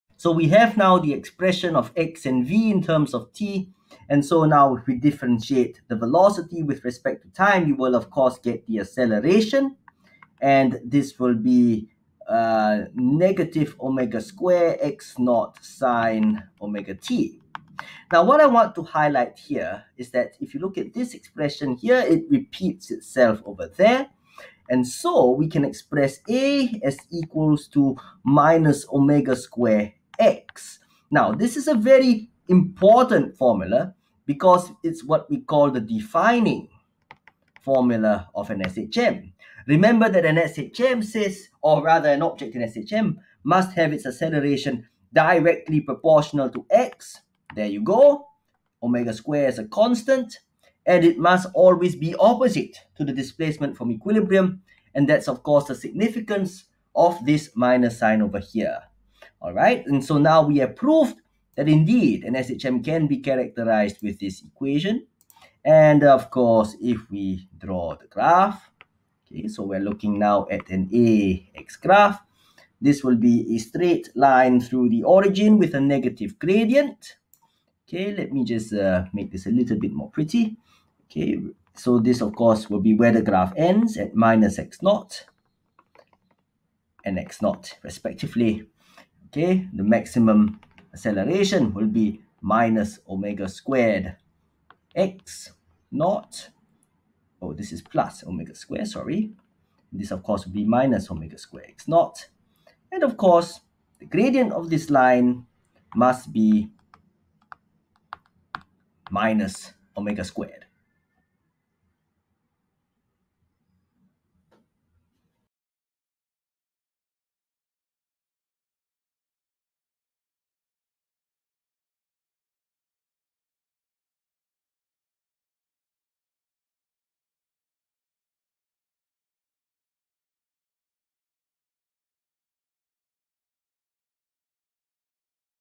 GCE A Level Physics | Acceleration – Displacement Relationship in SHM (Oscillations Chapter)
In the GCE A Levels, we must understand the relationship between an object’s acceleration and its displacement from equilibrium if it is in simple harmonic motion (SHM). We will derive the formulae and plot the graph describing this relationship.